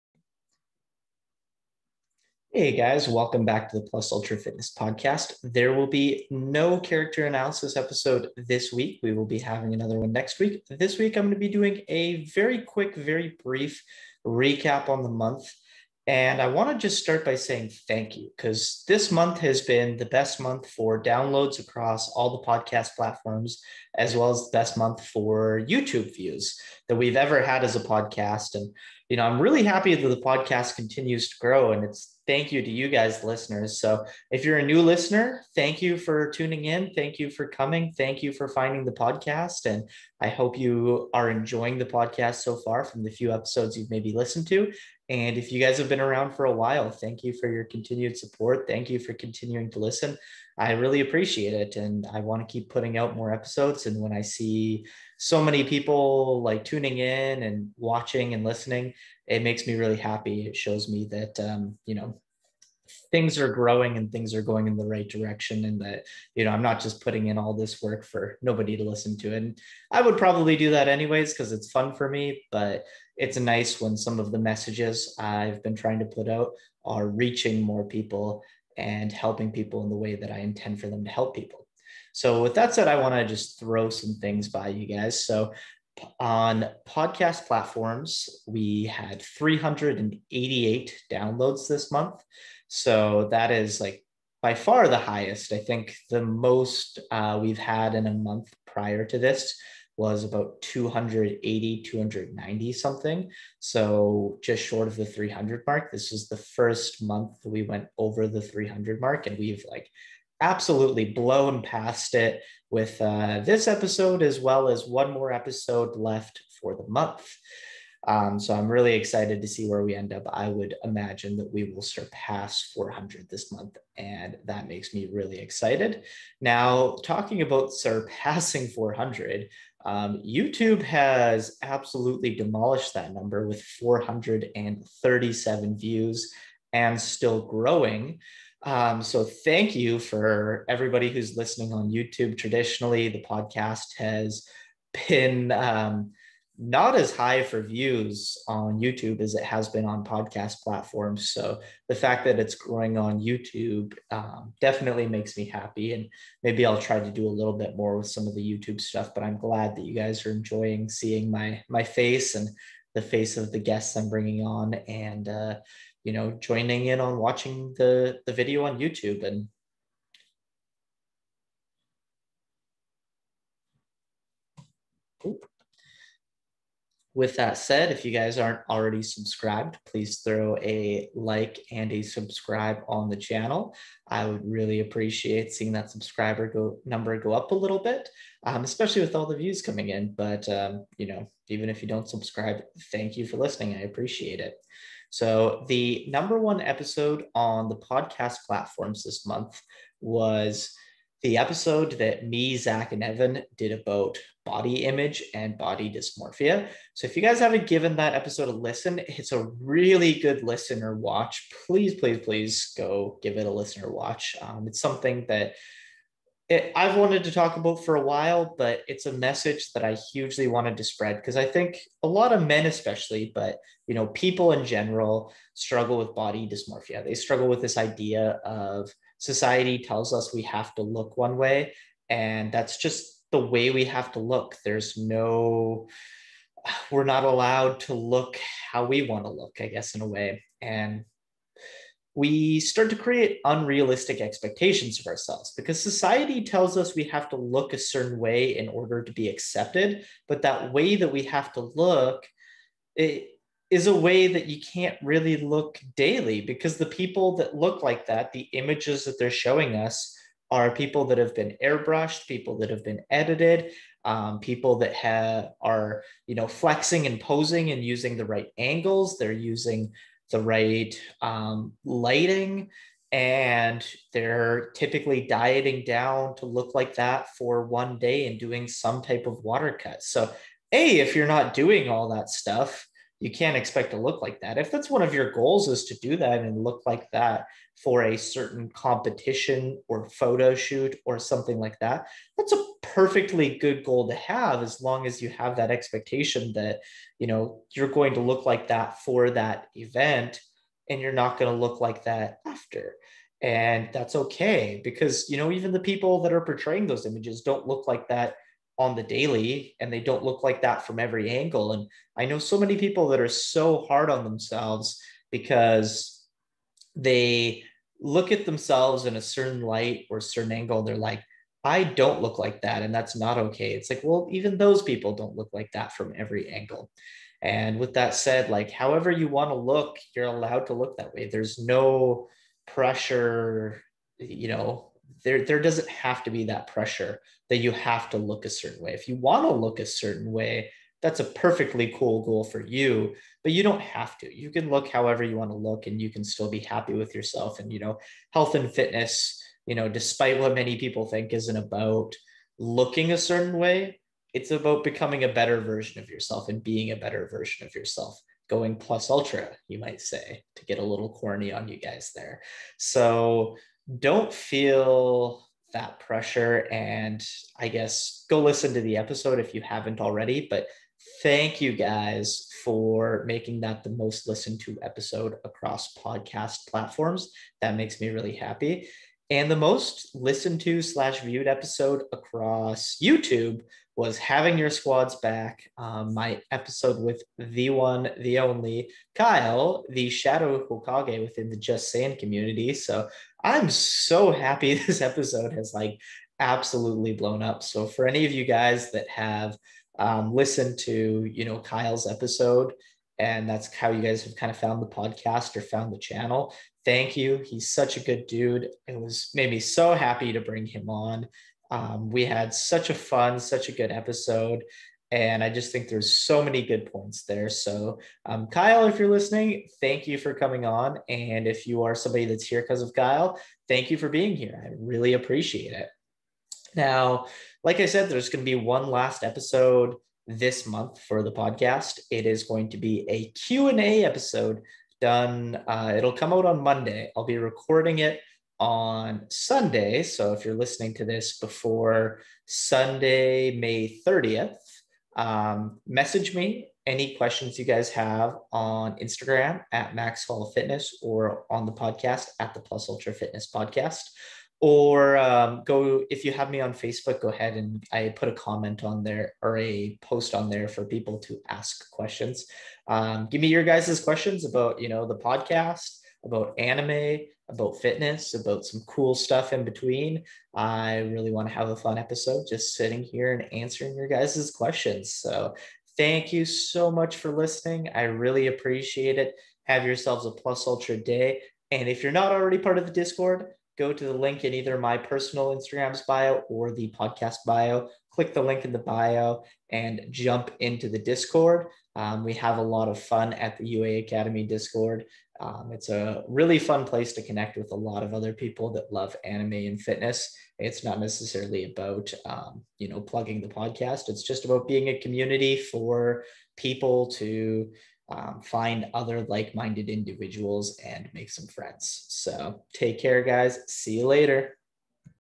May Recap | Ask Me Questions | Plus Ultra Fitness Podcast S2E17
GO PLUS ULTRA! Quick episode today, recapping the podcast highlights of May. Also drop questions below in the comments for Mondays Q&A.